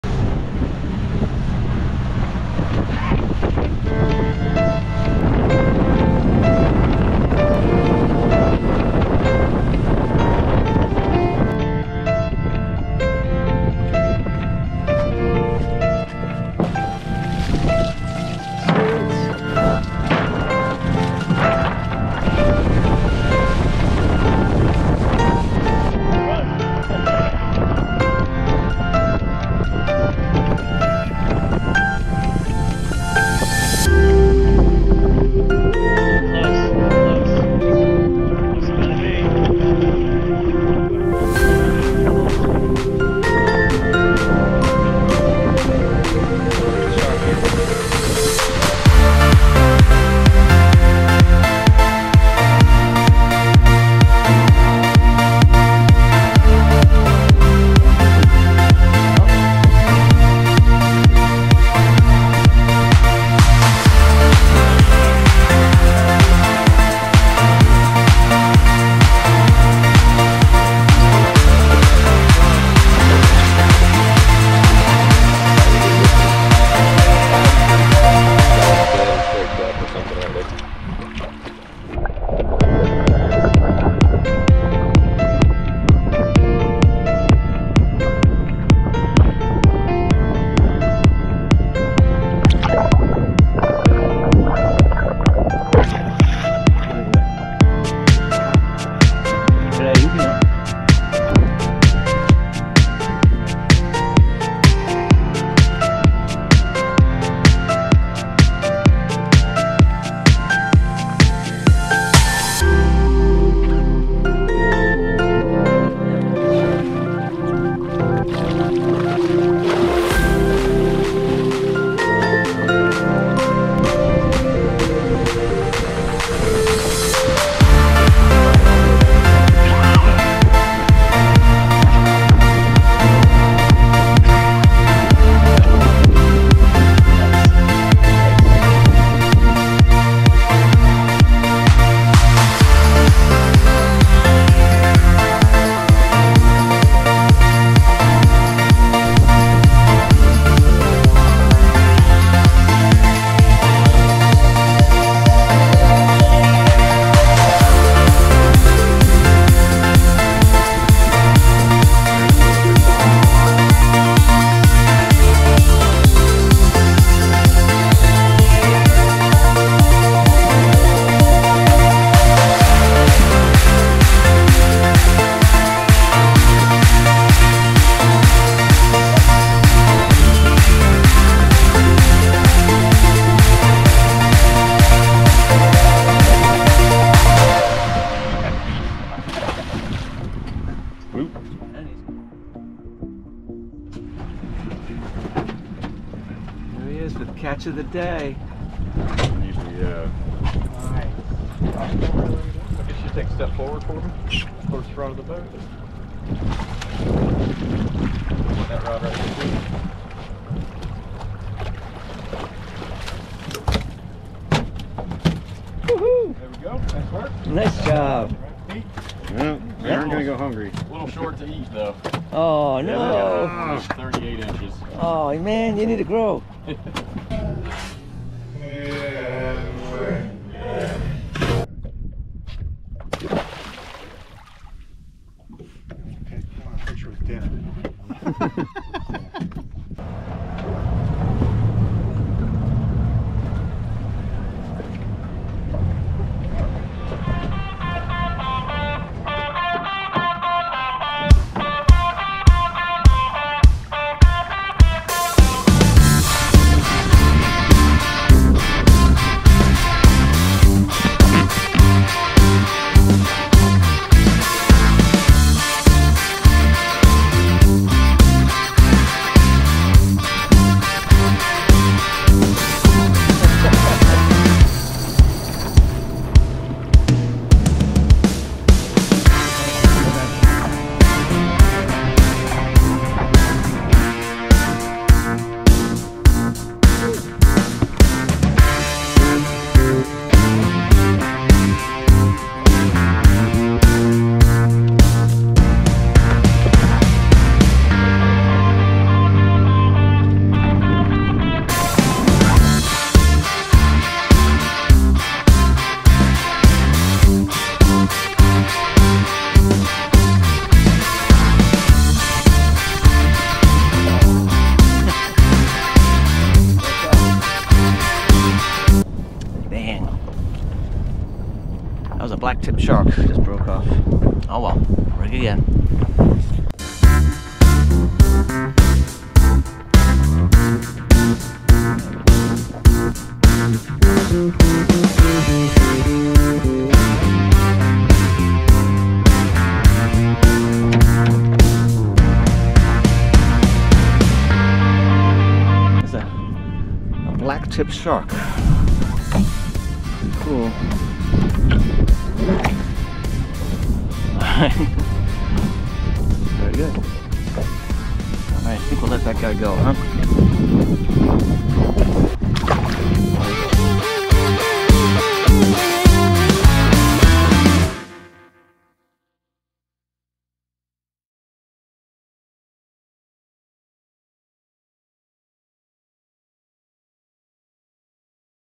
Hilton Head Shark Week!
I thought this was a fishing weekend with the guys ... but it turned out to be Shark Week! Two days of kayak fishing at Hilton Head NC, with a charter trip in the middle.